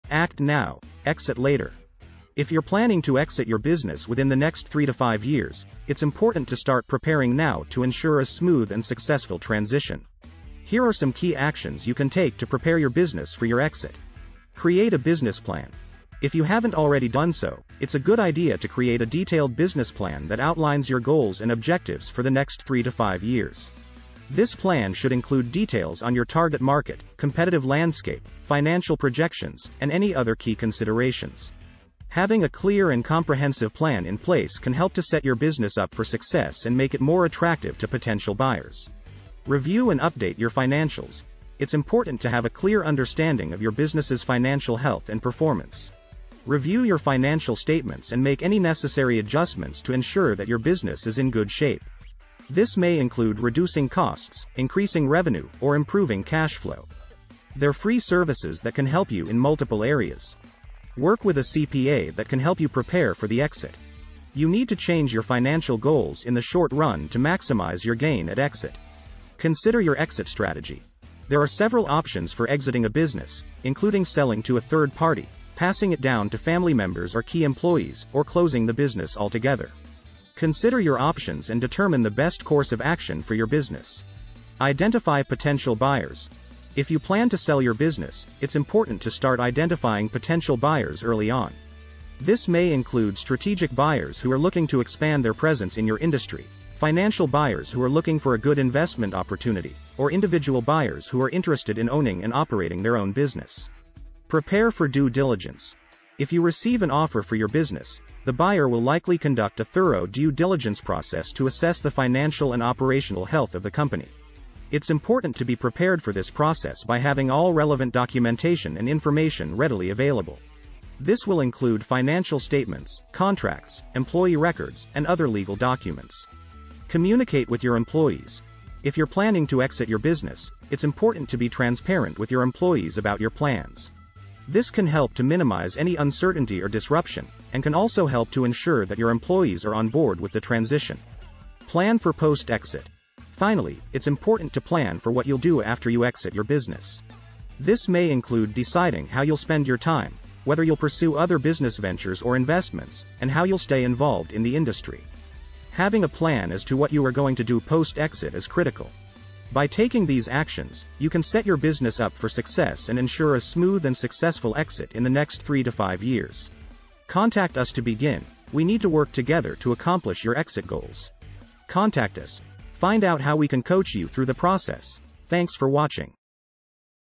Act Now - Exit Later - The Future Starts Now, John Stacy Business Advisor
If you're planning to exit your business within the next 3-5 years, it's important to start preparing now to ensure a smooth and successful transition. Here are some key actions you can take to prepare your business for your exit:

Create a business plan: If you haven't already done so, it's a good idea to create a detailed business plan that outlines your goals and objectives for the next 3-5 years. This plan should include details on your target market, competitive landscape, financial projections, and any other key considerations. Having a clear and comprehensive plan in place can help to set your business up for success and make it more attractive to potential buyers.

Review and update your financials: It's important to have a clear understanding of your business's financial health and performance. Review your financial statements and make any necessary adjustments to ensure that your business is in good shape. This may include reducing costs, increasing revenue, or improving cash flow. Work with a CPA that can help you prepare for the exit. You need to change your goals to maximize your gain at exit.

Consider your exit strategy: There are several options for exiting a business, including selling to a third party, passing it down to family members or key employees, or closing the business altogether. Consider your options and determine the best course of action for your business.

Identify potential buyers: If you plan to sell your business, it's important to start identifying potential buyers early on. This may include strategic buyers who are looking to expand their presence in your industry, financial buyers who are looking for a good investment opportunity, or individual buyers who are interested in owning and operating their own business.
Prepare for due diligence: If you receive an offer for your business, the buyer will likely conduct a thorough due diligence process to assess the financial and operational health of the company. It's important to be prepared for this process by having all relevant documentation and information readily available. This may include financial statements, contracts, employee records, and other legal documents.

Communicate with your employees: If you're planning to exit your business, it's important to be transparent with your employees about your plans. This can help to minimize any uncertainty or disruption, and can also help to ensure that your employees are on board with the transition.

Plan for post-exit: Finally, it's important to plan for what you'll do after you exit your business. This may include deciding how you'll spend your time, whether you'll pursue other business ventures or investments, and how you'll stay involved in the industry.

By taking these actions, you can set your business up for success and ensure a smooth and successful exit in the next 3-5 years.

Expert executive business coach in The Woodlands, Conroe, and Spring
CatalystStrategyConsulting.com